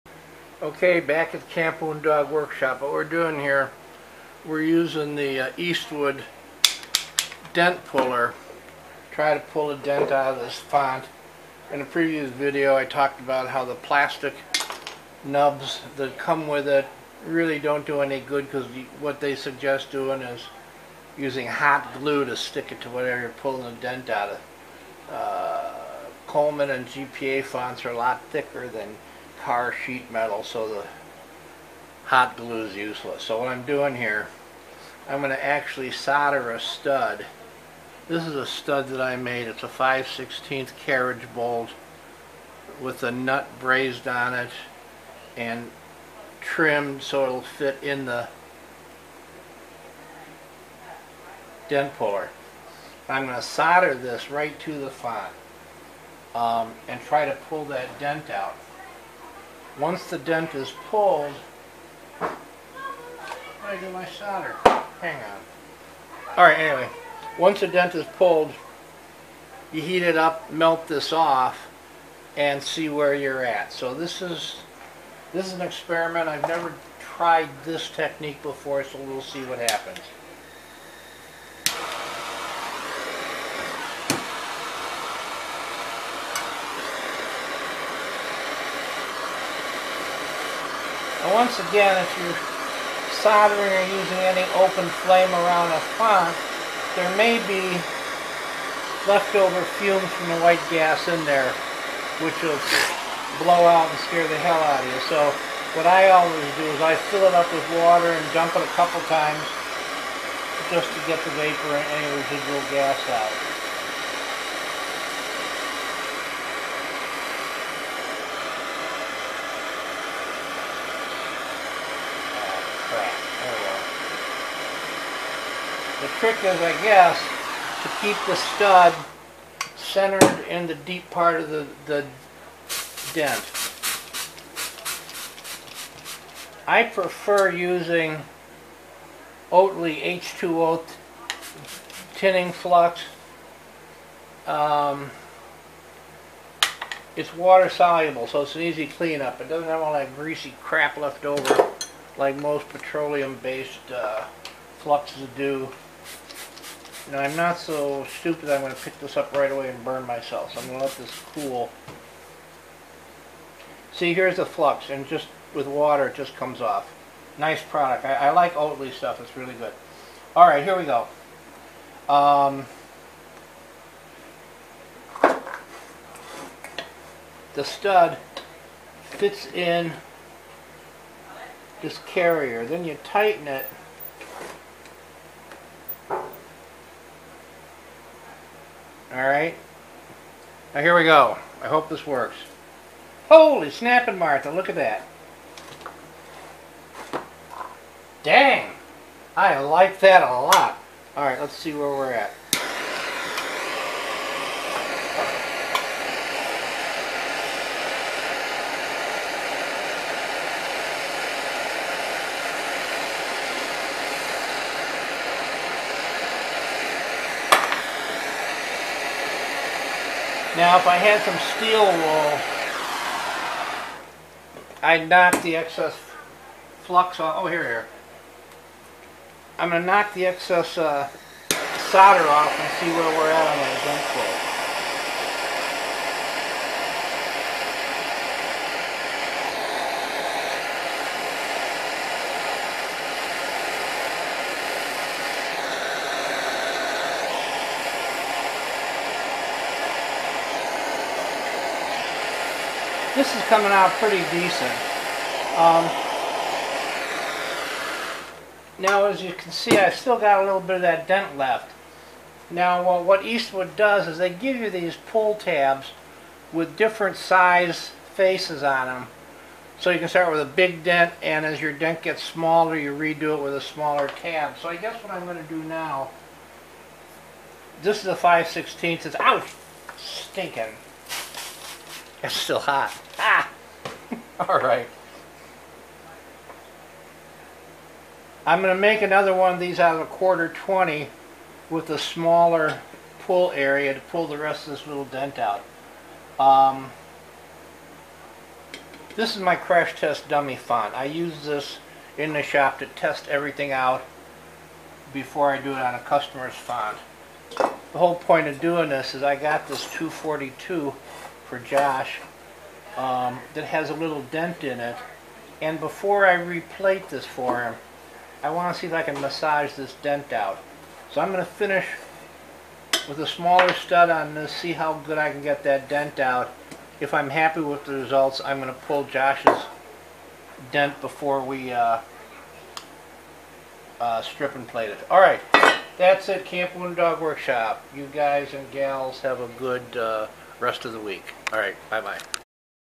Camp Wounded Dog Workshop using Eastwood dent puller on a font
with a minor modification of parts the Eastwood dent puller is the cats behind for pulling dents out of fonts-- this is the first try- and I like it